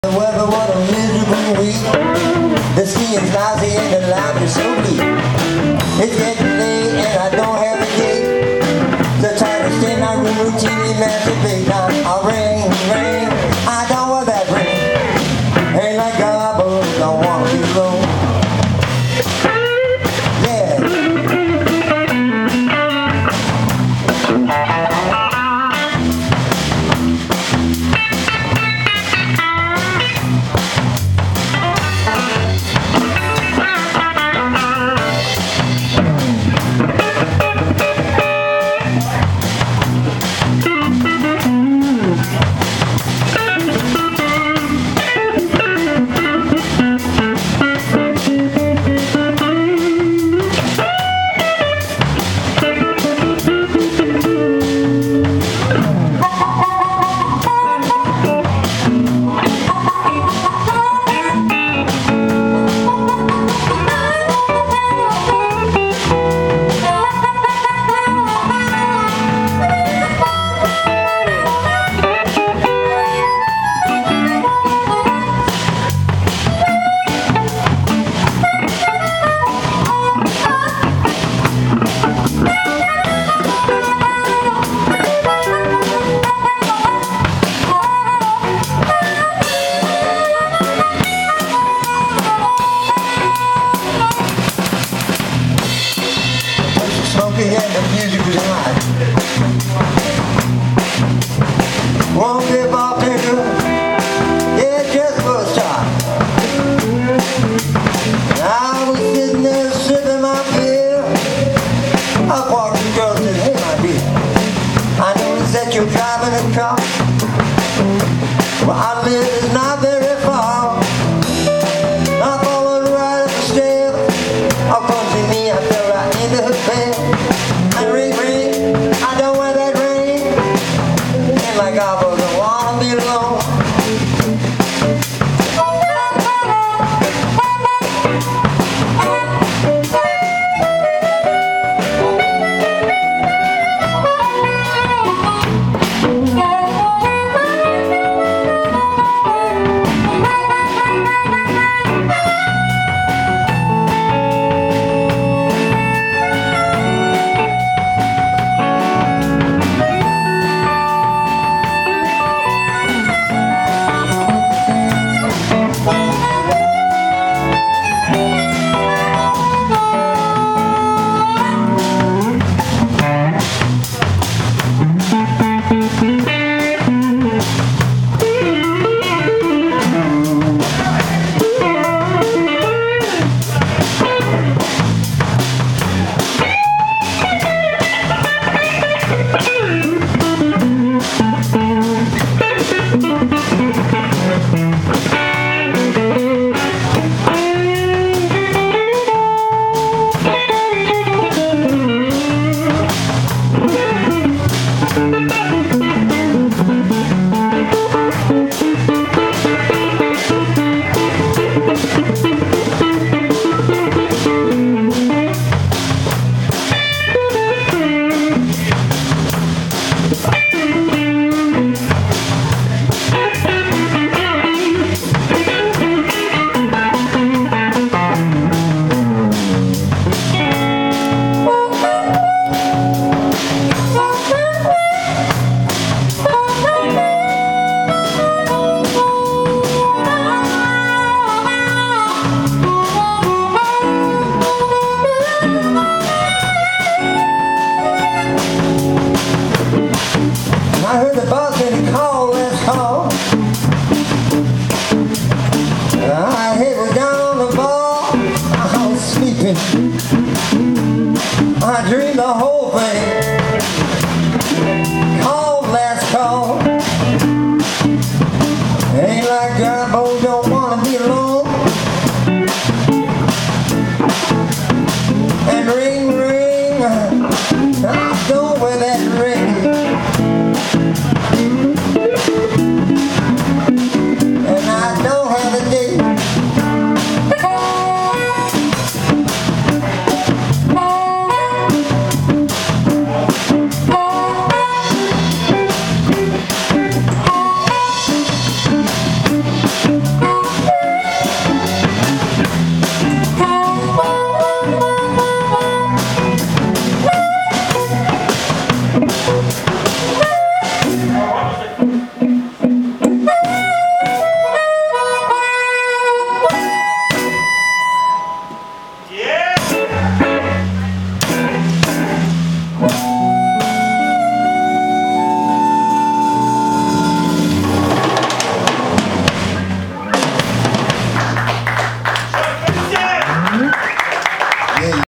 Chuck Vincent and Amil
Chuck Vincent-guitar, Amil-harmonica/vocals, Robert Conti-bass, Mike Harris-drums